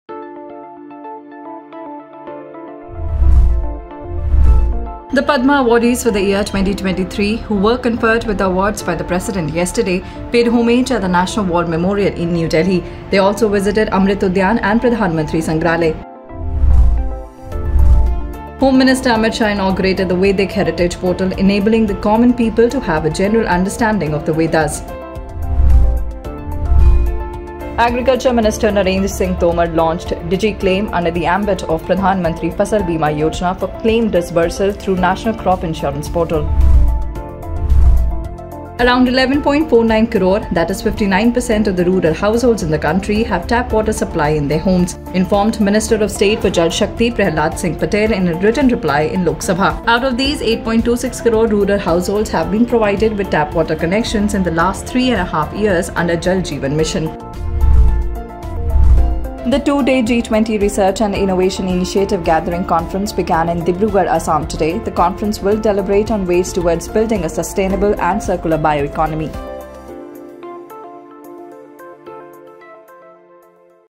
DailyWrap | 23-03-2023
💠Union Home Minister Amit Shah inaugurated Vedic Heritage portal

💠Union Minister N S Tomar launched DigiClaim under Pradhan Mantri Fasal Bima Yojana